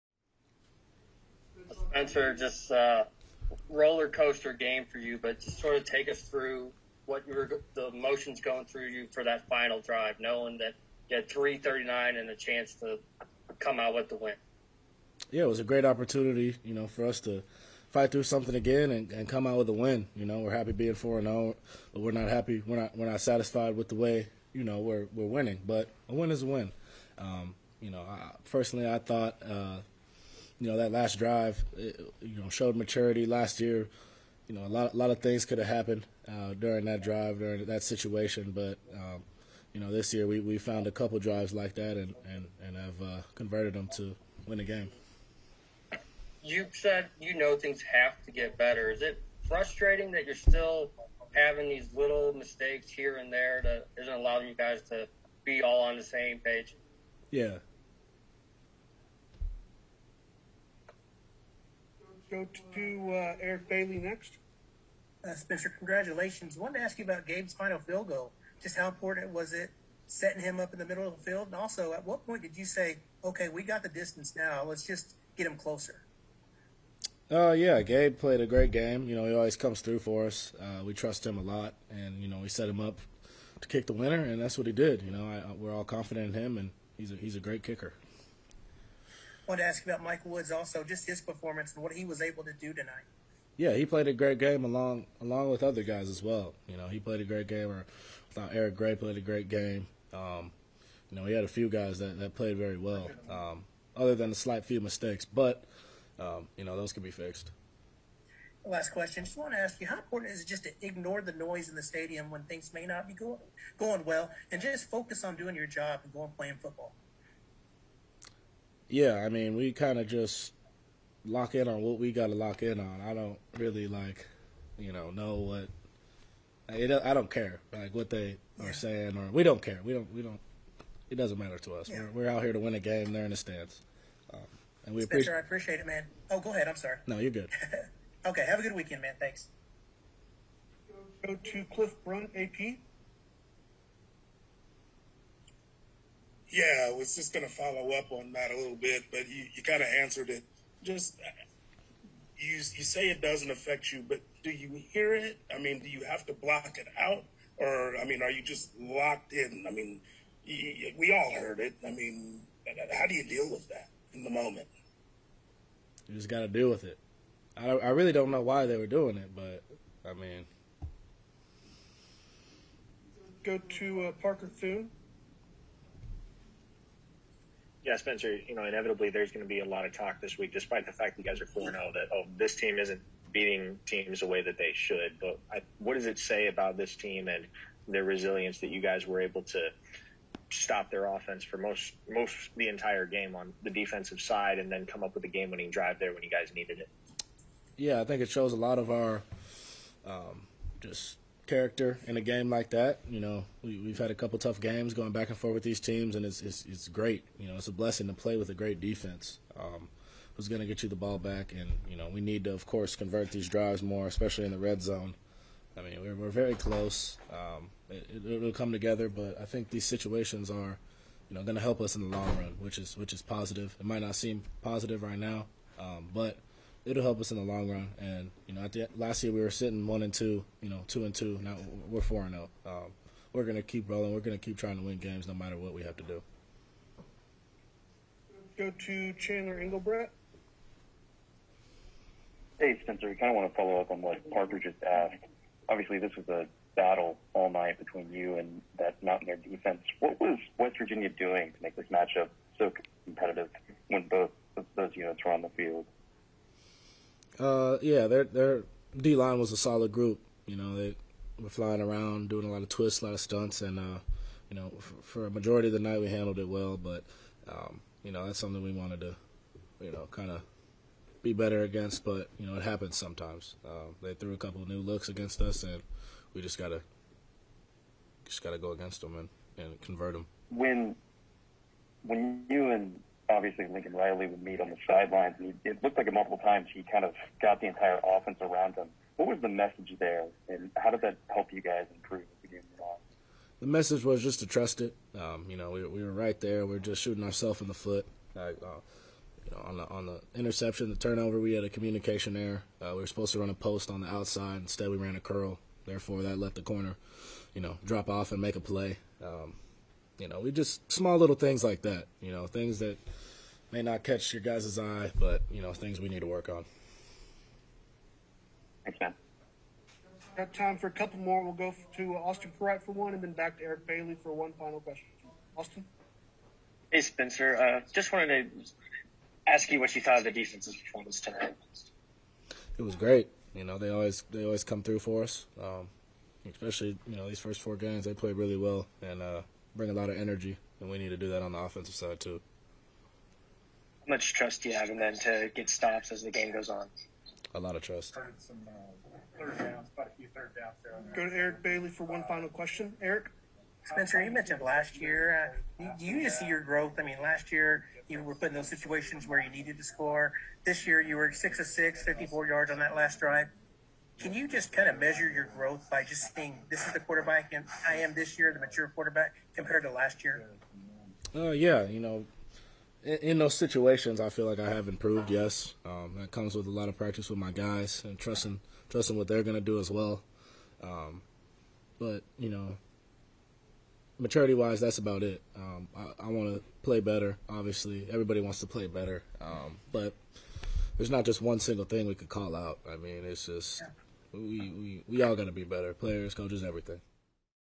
Spencer Rattler WVU Postgame
Oklahoma Sooners quarterback Spencer Rattler meets the press after the Sooners' 16-13 win over West Virginia on Saturday, Sept. 25, 2021.